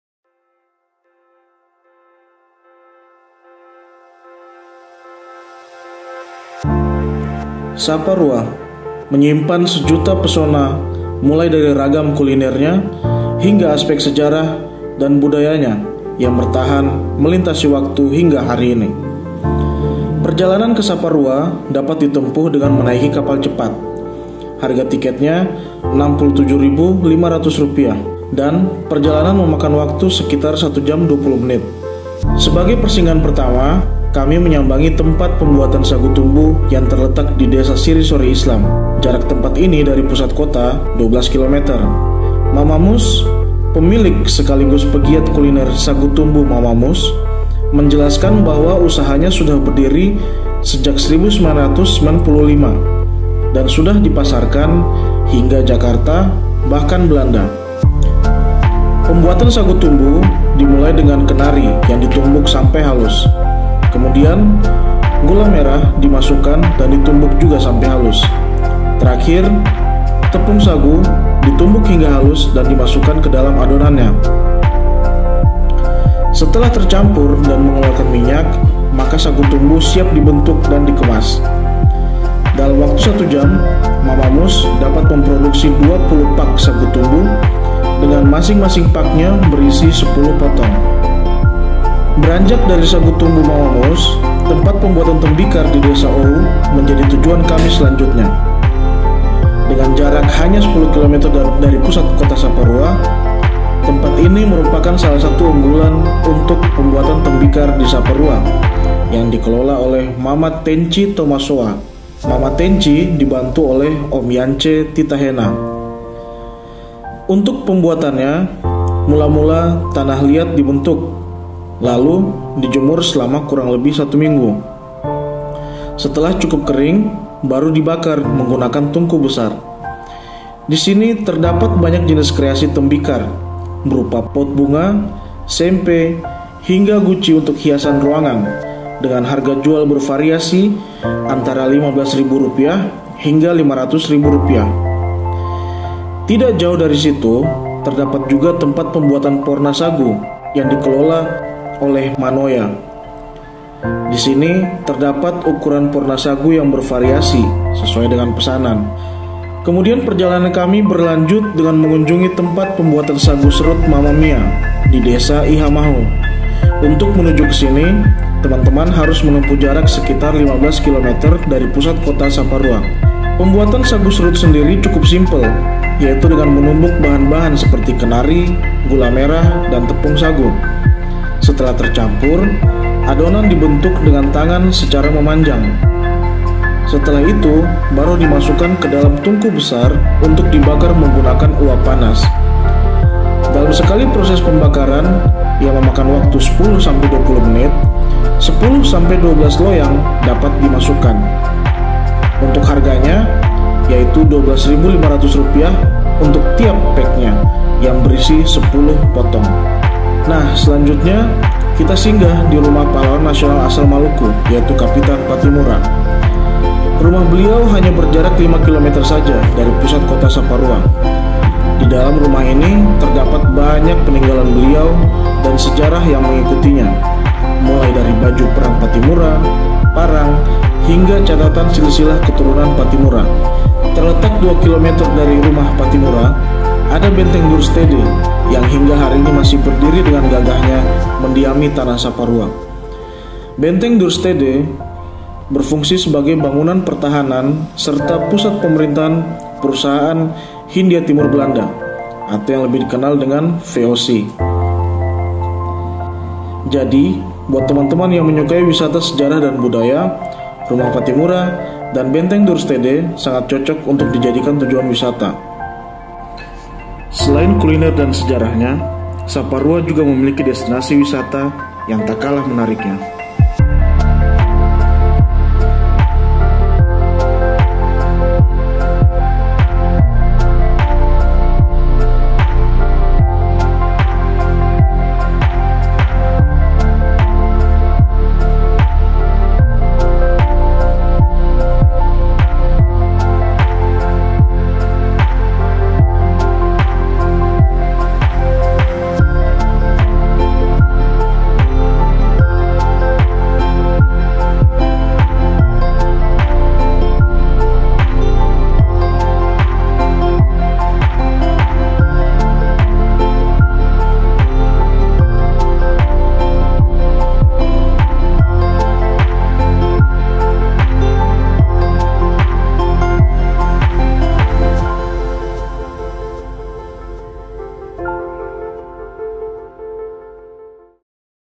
Cerita dari Saparua
Tayangan video ini merupakan rangkuman perjalanan selama 3 hari di Saparua, mulai dari kuliner khas Saparua seperti sagu tumbu, serut sagu, dll hingga keindahan destinasi wisatanya.